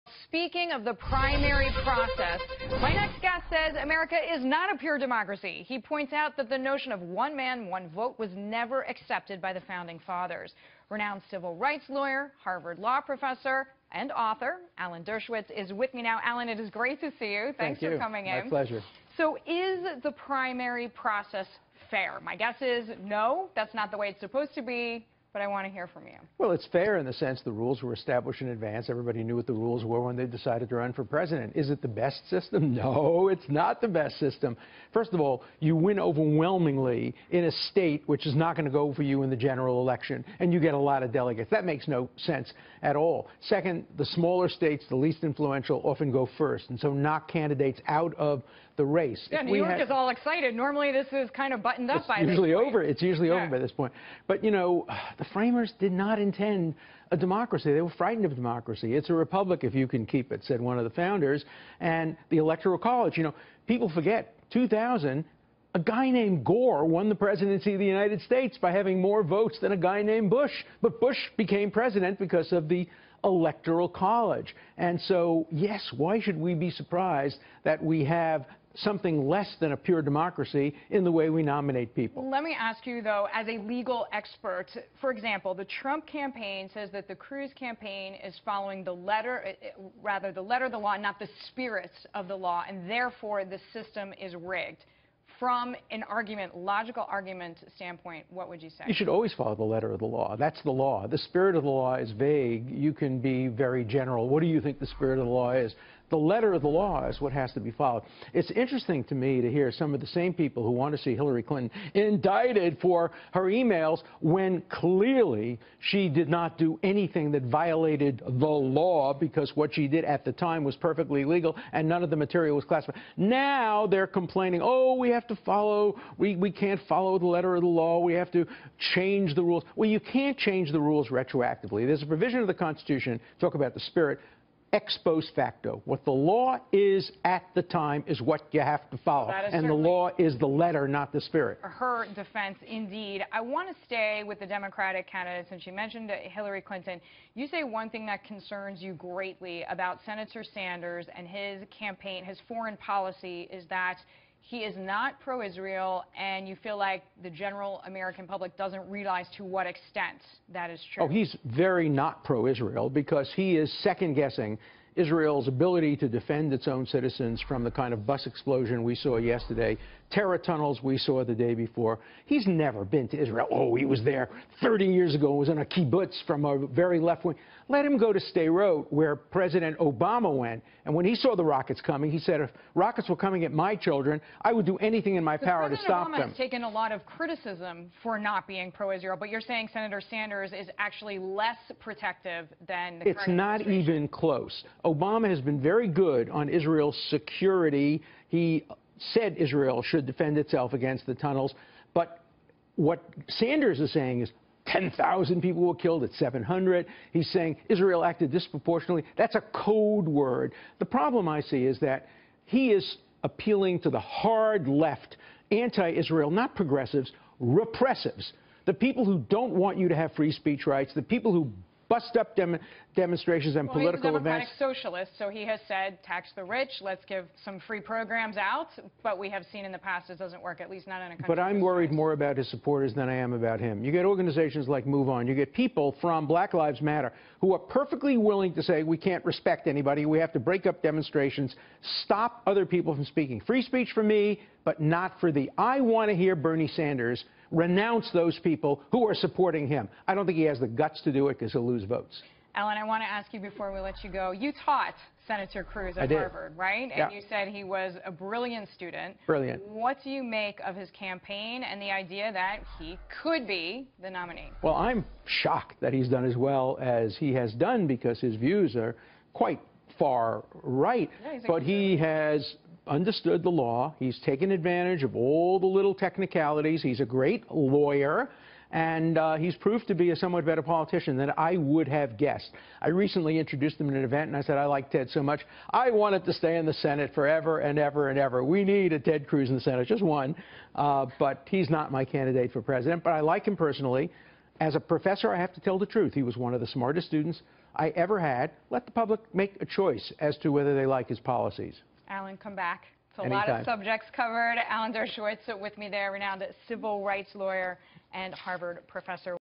Dershowitz: I’m more worried about Sanders’ supporters than I am about him
Harvard Law Professor and Author Alan Dershowitz on the primary process and Bernie Sanders’ policy for Israel.